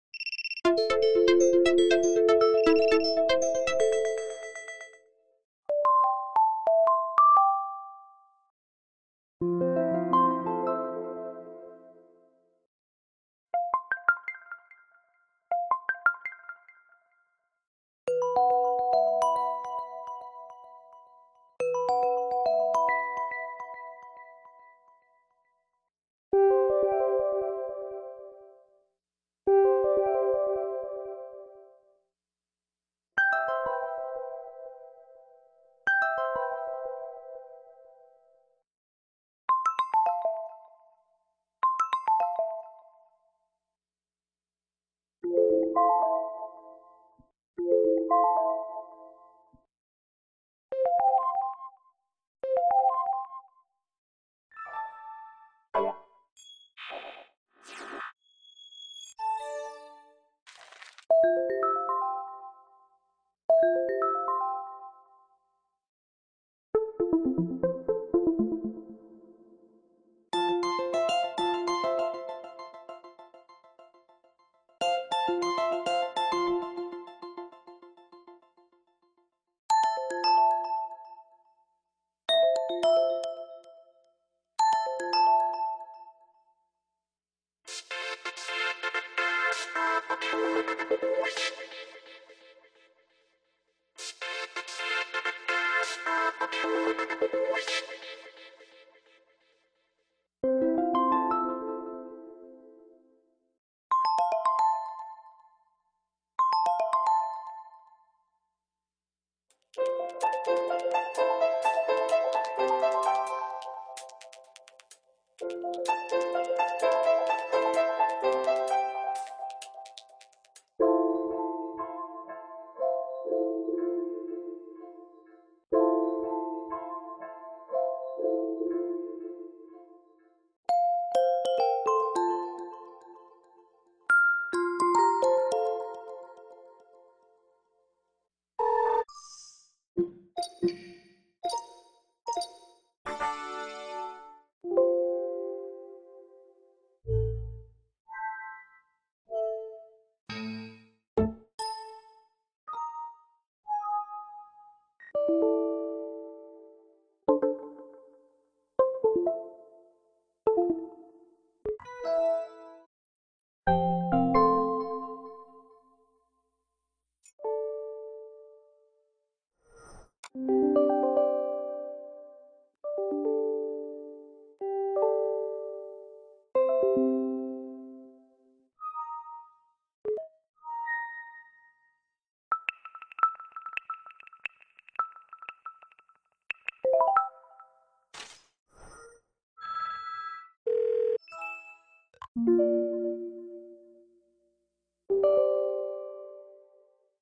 All Windows 10 Sounds
some sounds sound like from windows vista and windows 7
wow some we never here like startup and shutdown sounds